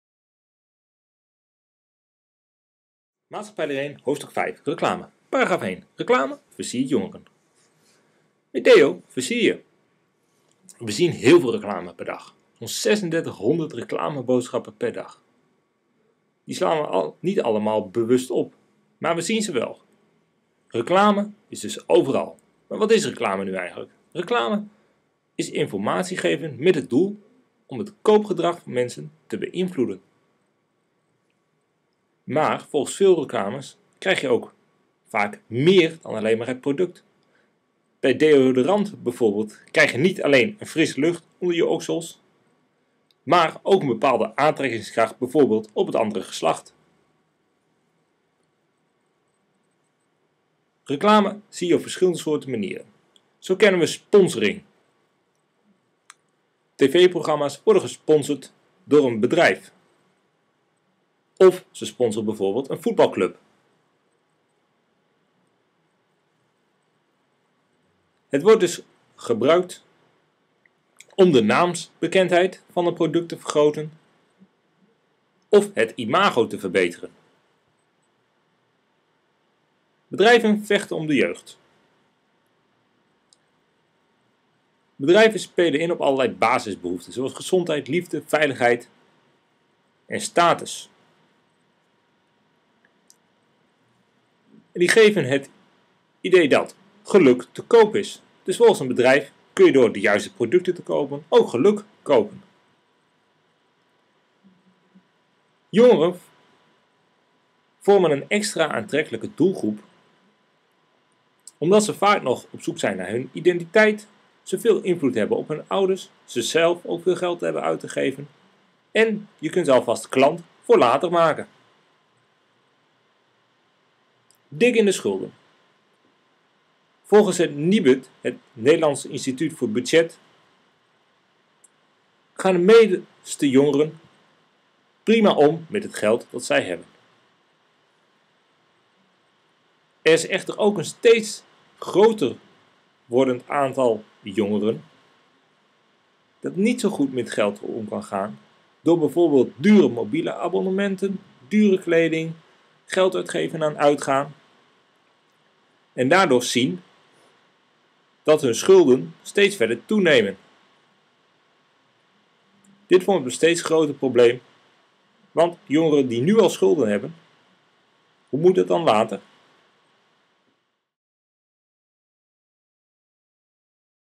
H5 §1 Met deo versier je!
De uitleg bij Maatschappijleer 1, Hoofdstuk 5, Reclame.
§1: Met deo versier je!